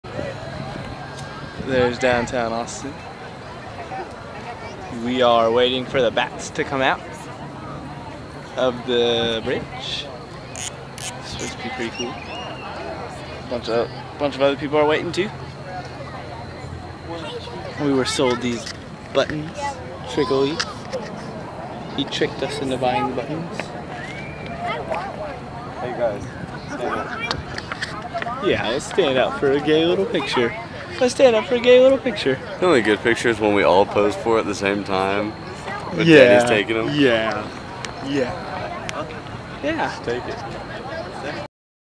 Texas Entry 44
This is Me, Kyle, and Trevor's trip to Austin Texas!